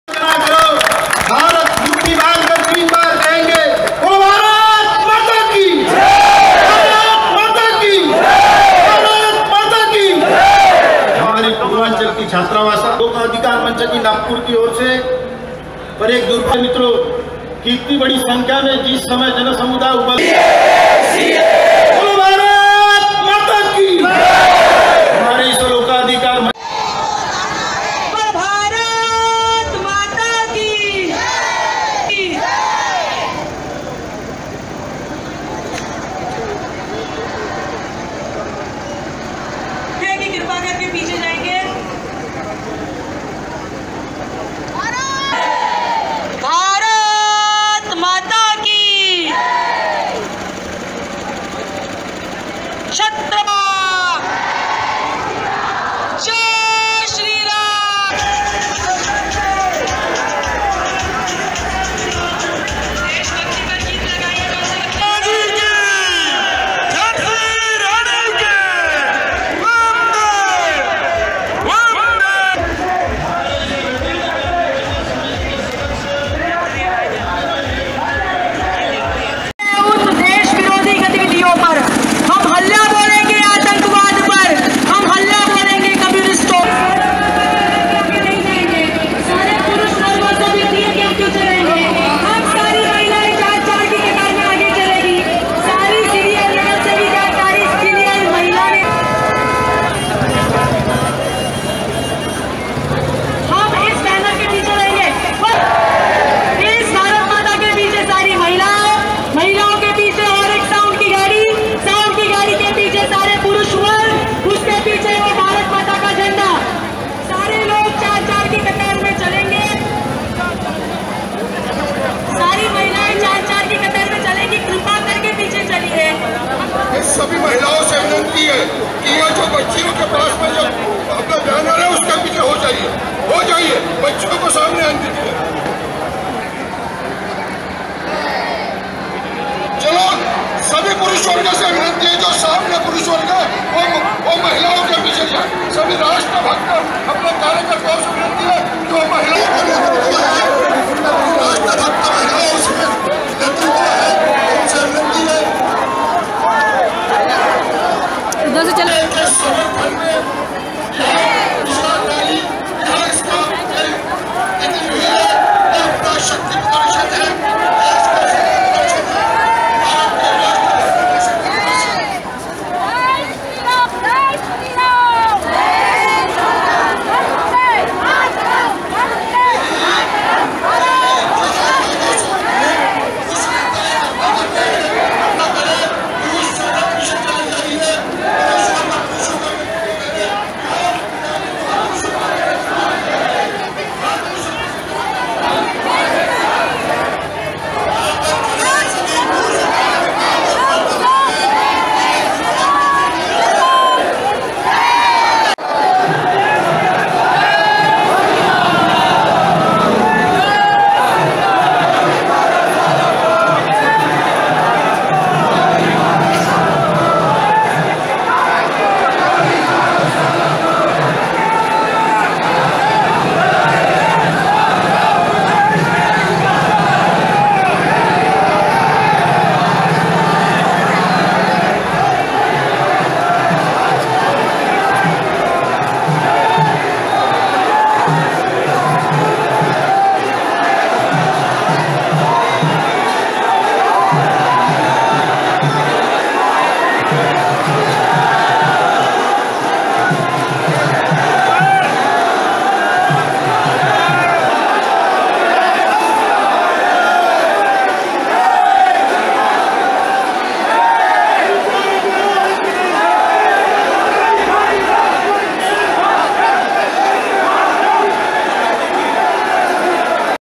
नागपुर मध्ये भाजपचा CAA आणि NCR च्या समर्थनात भव्य मोर्चा; केंद्रीय मंत्री नितीन गडकरी उपस्थित
नागपुर मध्ये भाजपचा CAA आणि NCR च्या समर्थनात भव्य मोर्चा काढण्यात आला आहे.  या मोर्च्या मध्ये  केंद्रीय मंत्री नितिन गडकरी व अनेक कार्यकर्ते उपस्थित आहेत.